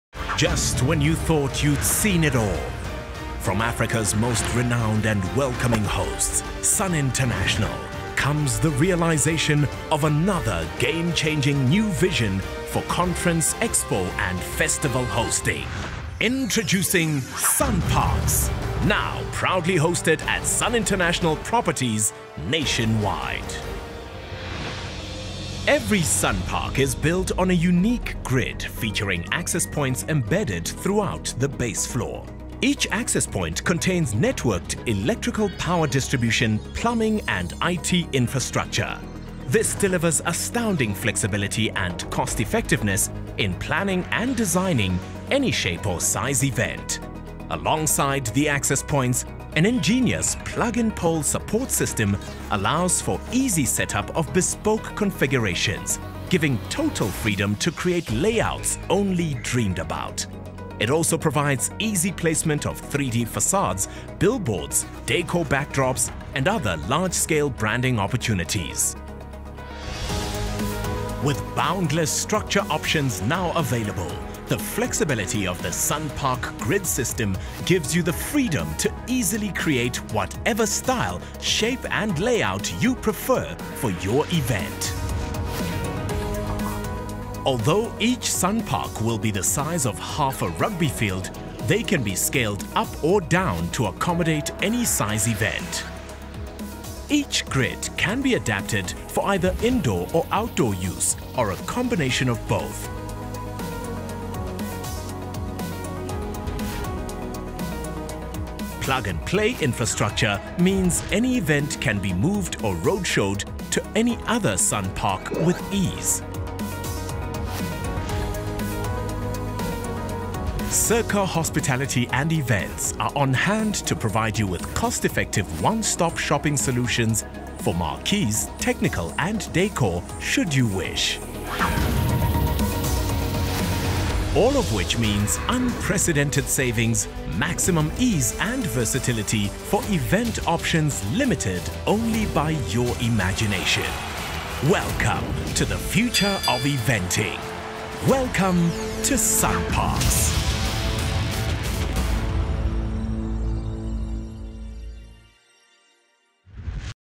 Sun Park | Get On The Grid! | Sun International
Inspired by the growing need for bespoke, interactive and specialised event concepts and brand activation initiatives, our Sun Parks function venues have been designed as game-changers in the industry and offer unparalleled flexibility, scalability, convenience and cost-effectiveness.